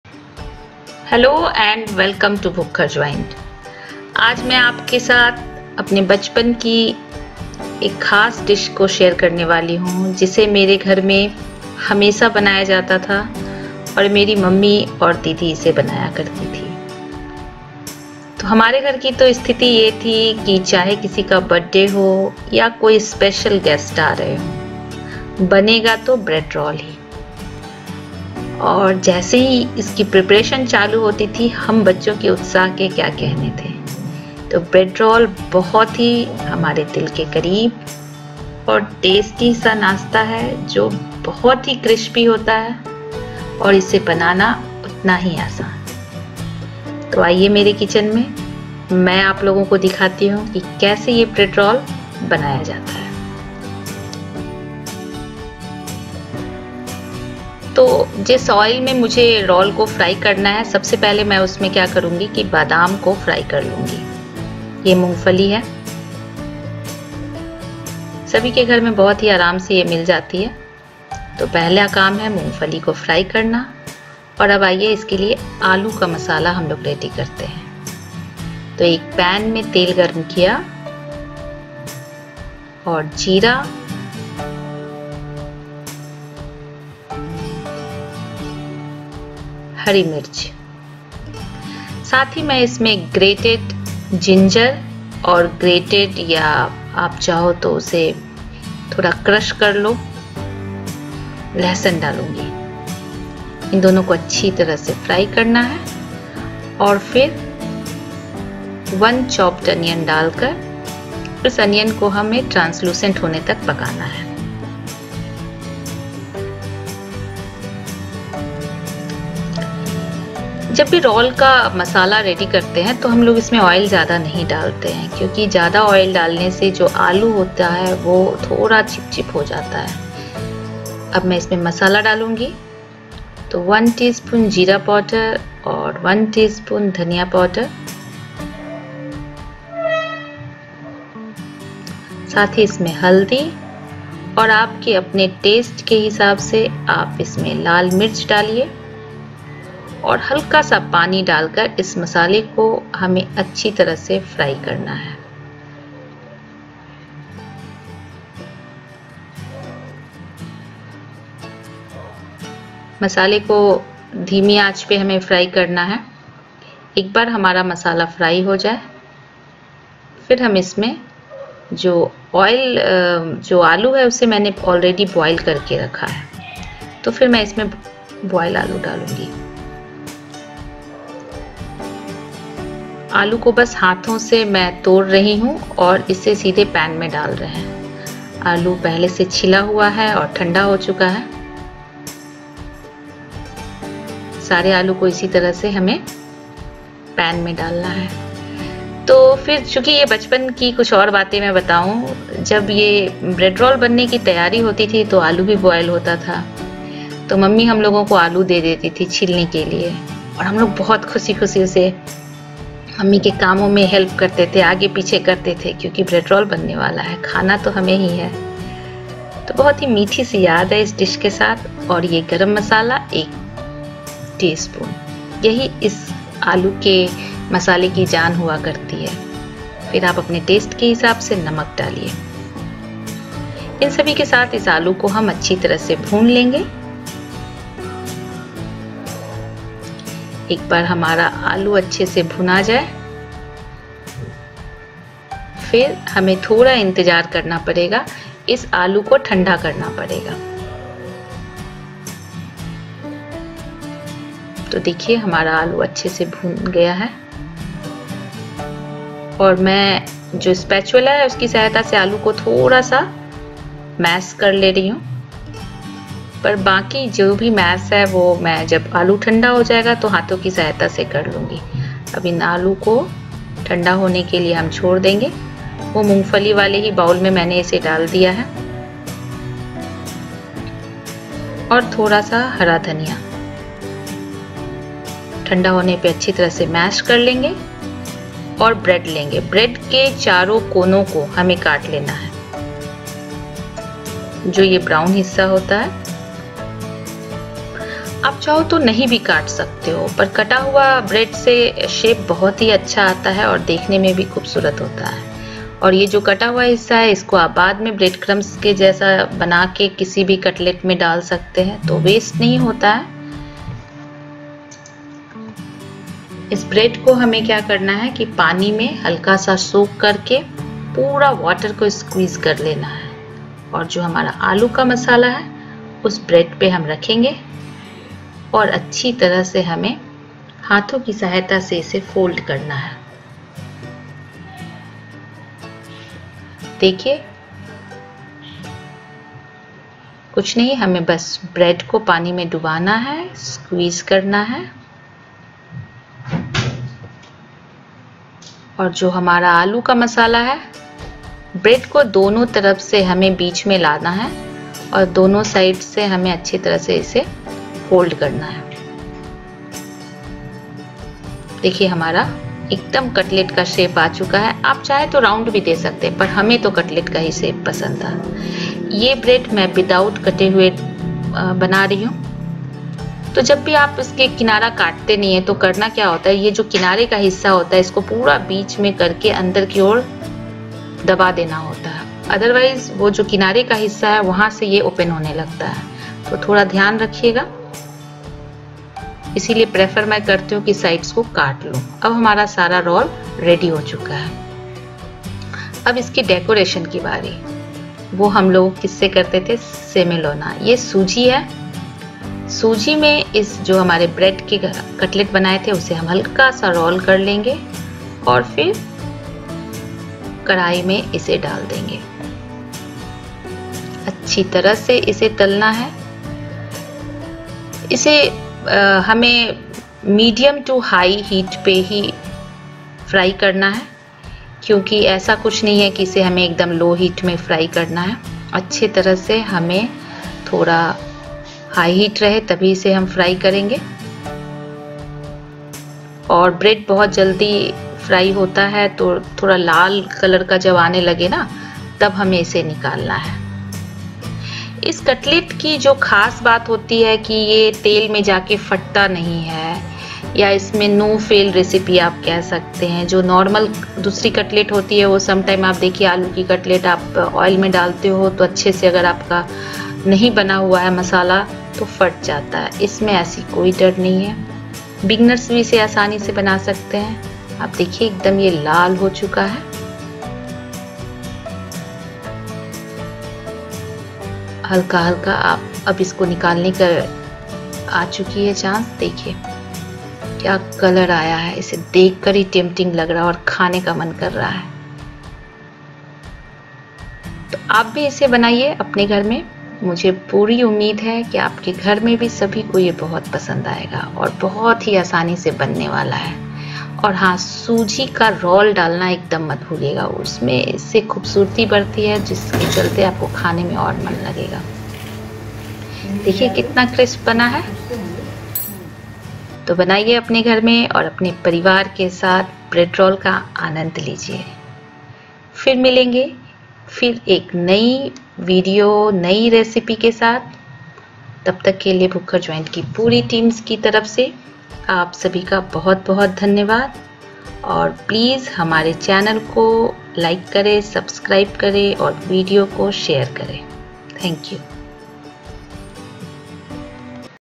Bread roll /evening nasta / shaam ka nasta/snacks
Bread roll is a tasty snack that can be made at home easily  if you have leftover boiled potatoes. These bread roll are a favorite at home and usually pair them with the evening tea as a snack.

bread patties,bread roll, cutlet, u can name it any way .best suited for evening nasta.